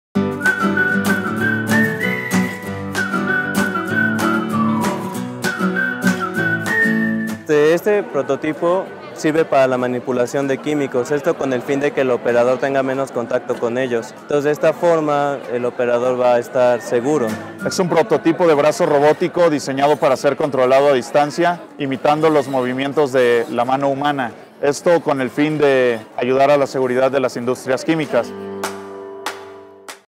Prototipo de brazo robótico
📹Los alumnos de Ingeniería Mecatrónica tienen entre sus proyectos un prototipo de brazo robótico, que sirva de apoyo a la industria química.